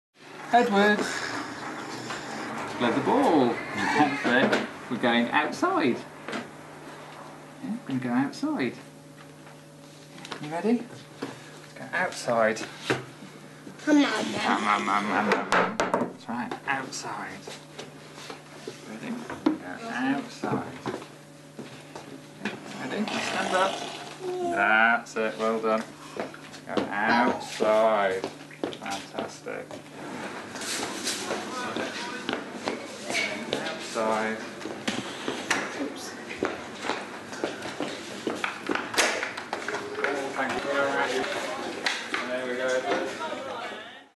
AAC Edward objects of reference
In this film the teacher is showing Edward a ball as a reference to communicate outside. Notice how right from the beginning the teacher is helping Edward to see this communication aid is shared between us by the shared holding of the ball. In this way Edwards may in the future know to use the ball to ask if he can go outside.